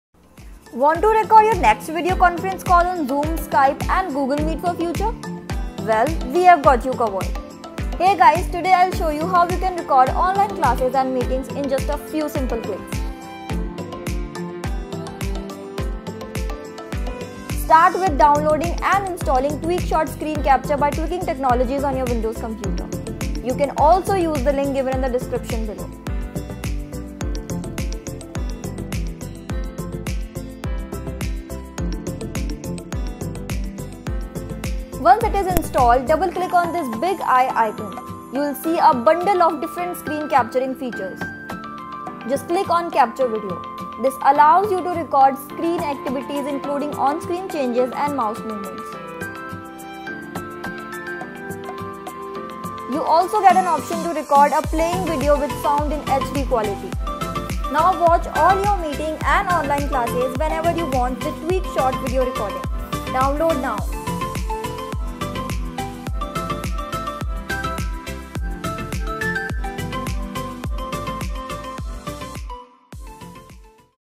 How to Record Any online Meeting (Audio + Video) on ( PC + Laptop )
The world is changing, everything from office work to education is switching to live stream, video call, etc. Therefore, to make most of these online sessions and to ensure you don’t miss out on any important information, recording online sessions is best. Here in this video, we explain how you can
record online sessions using simple software – TweakShot.

As the name explains this software takes a screenshot of still images and lets you record the screen too. Not only this, using it you can edit the images, trim video, add annotations, highlight images, and do a lot more. To use TweakShot download it and watch the video till the end.